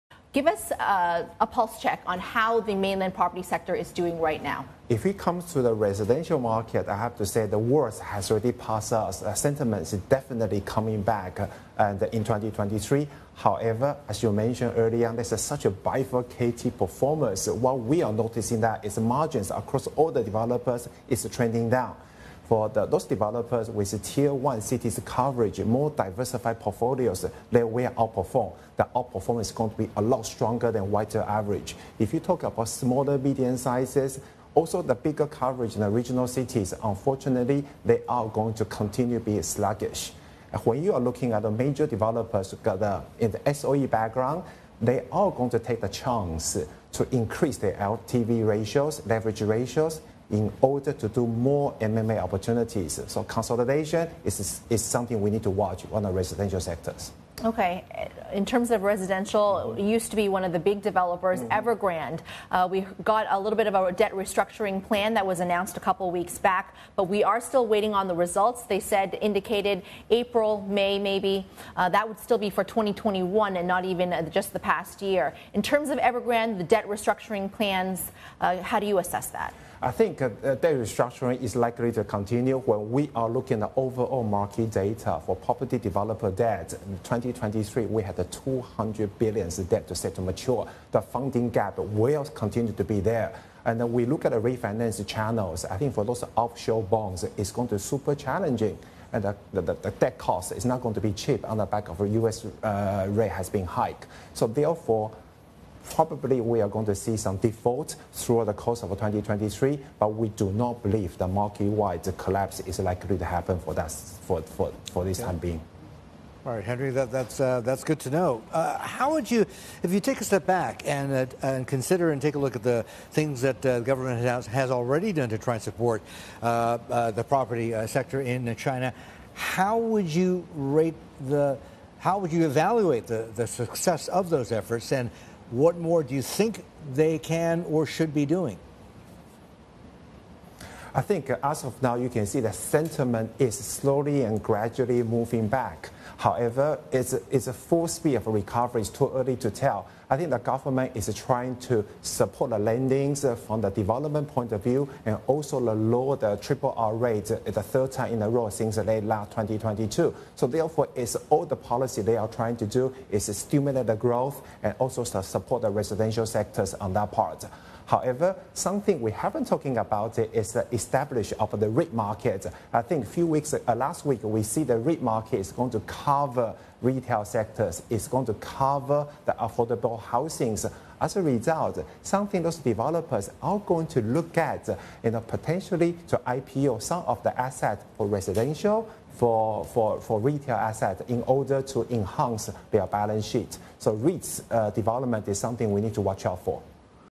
The performance of China's residential market is 'bifurcated,' research firm
Henry Chin of CBRE Research says the worst for the country's property sector is over and "sentiment is definitely coming back in 2023," but performance is uneven.